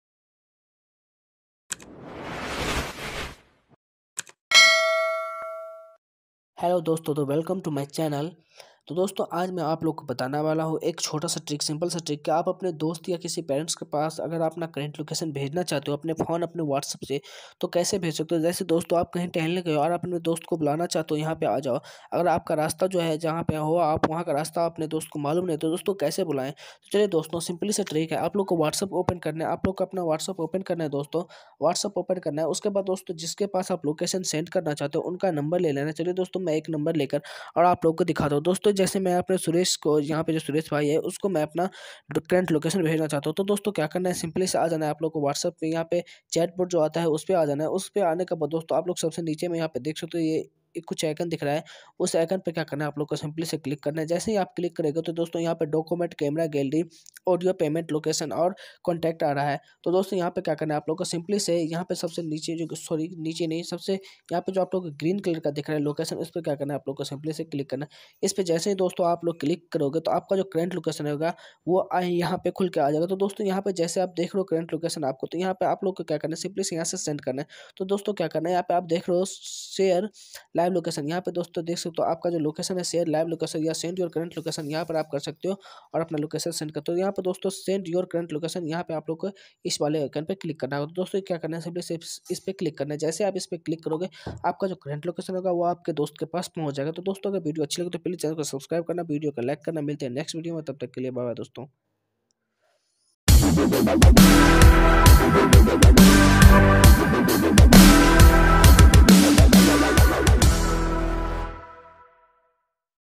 WhatsApp se kaise kisi location bhenje अपना location किसी को कैसे भेजे
Apne WhatsApp se kaise kissi ko apna location bhenje
apne WhatsApp par
kisi ko bhi

about my this channel other video link